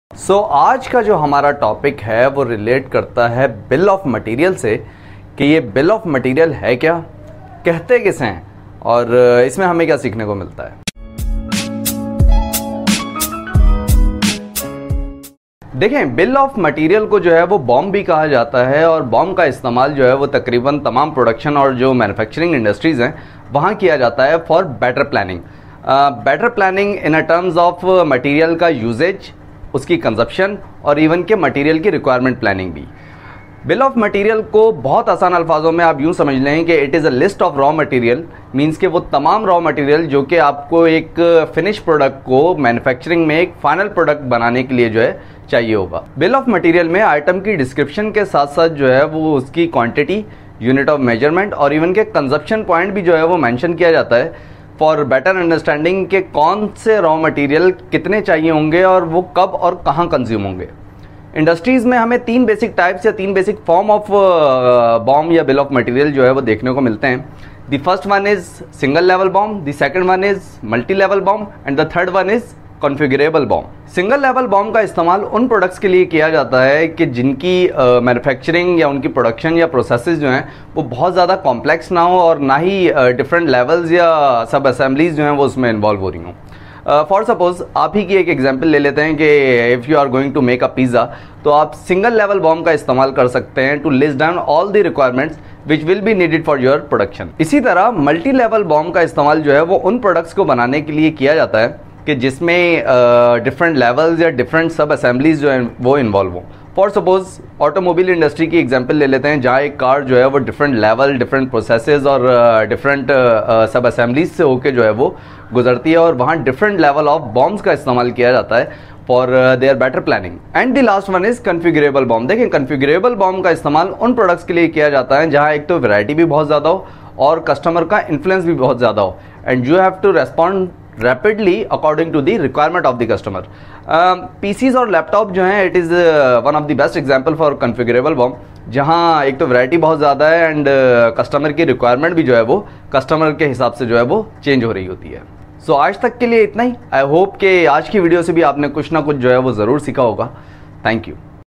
Bill Of Material - BOM Example - Types of BOM - Urdu-Hindi-SCM
Bill of material is also called BOM.
BOM is a detail description of material which is required in production & manufacturing.
There are 3 main types of BOM
1: Single Level BOM
2: Multi Level BOM
3: Configurable BOM